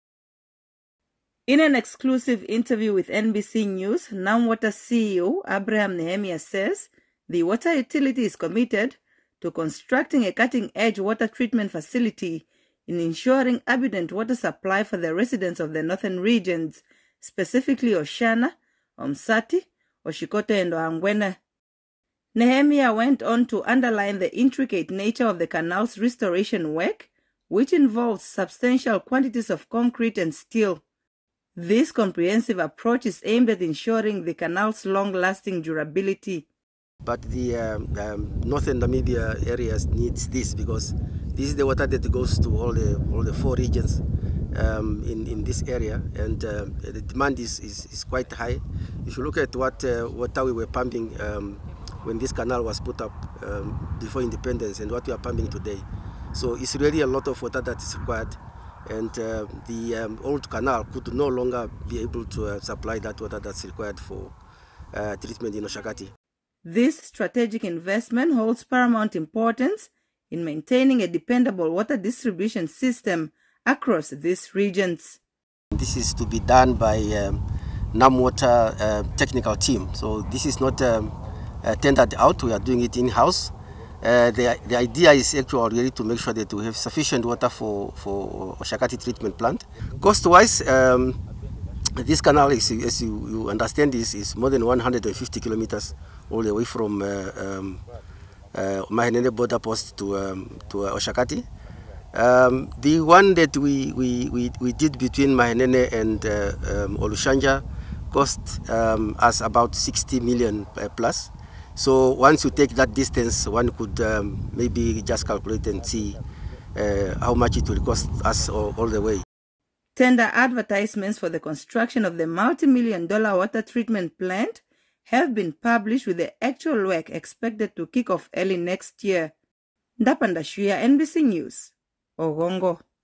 NamWater to rehabilitate canal in the North - nbc
NamWater is working on the rehabilitation of its canal in an effort to ensure that the northern communities have access to an ample supply of clean drinking water.